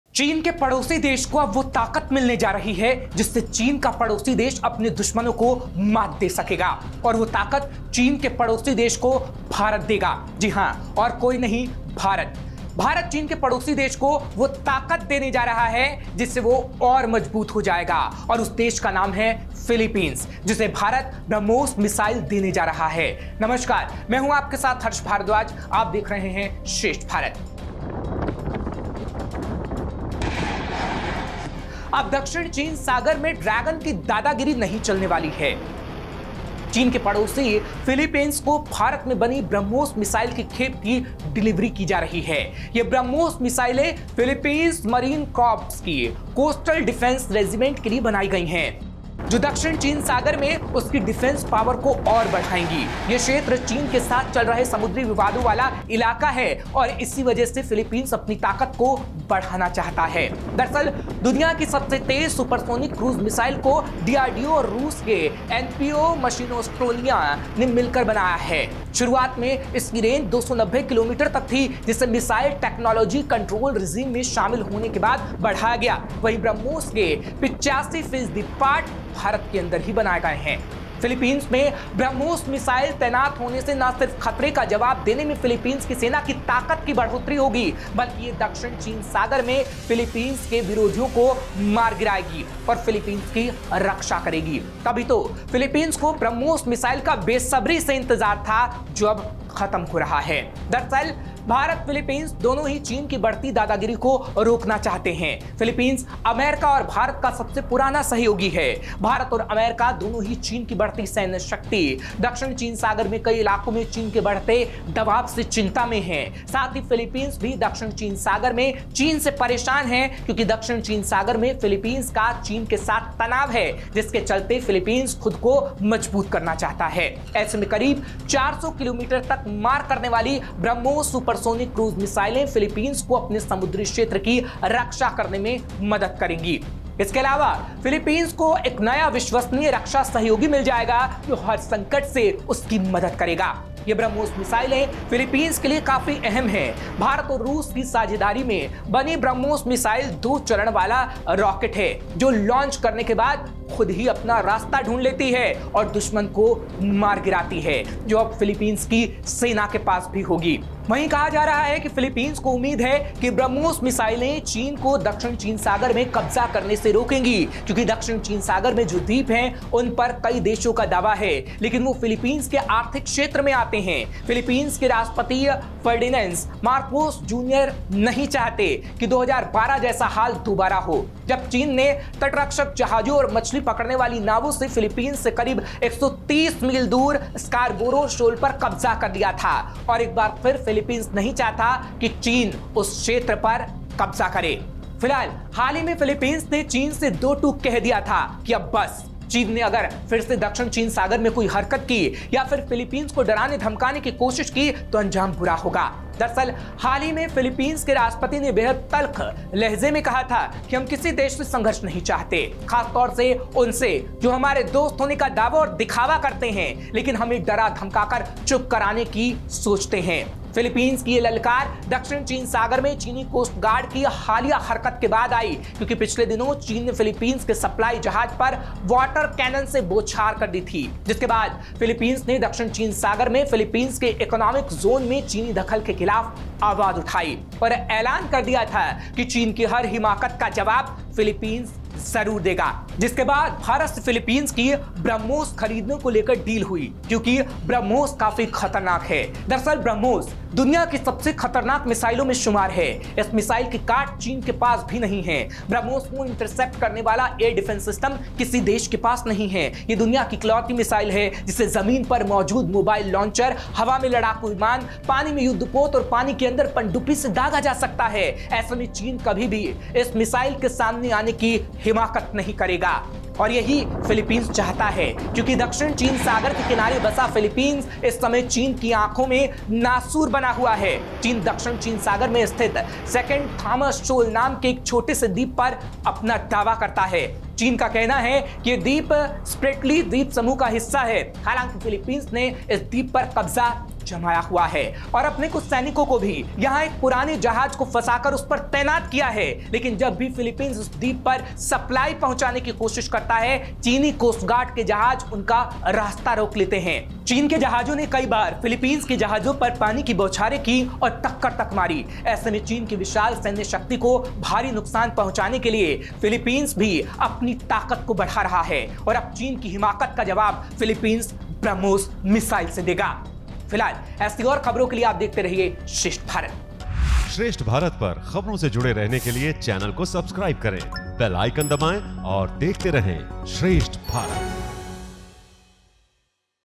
Brahmos China को सिखाएगी सबक, Philippines से China होगा परास्त
अब दक्षिण चीन सागर में ड्रैगन की दादागीरी नहीं चलने वाली है. चीन के पड़ोसी फिलीपींस को भारत में बनी ब्रह्मोस मिसाइल की खेप की डिलीवरी की जाएगी. ये ब्रह्मोस मिसाइलें फिलीपींस मरीन कॉर्प्स की कोस्टल डिफेंस रेजिमेंट के लिए बनाई गई हैं, जो पश्चिम फिलीपींस सागर में उसकी डिफेंस पावर को बढ़ाएंगी. यह क्षेत्र चीन के साथ चल रहे समुद्री विवादों वाला इलाका है, और इसी वजह से फिलीपींस अपनी ताकत को बढ़ाना चाहता है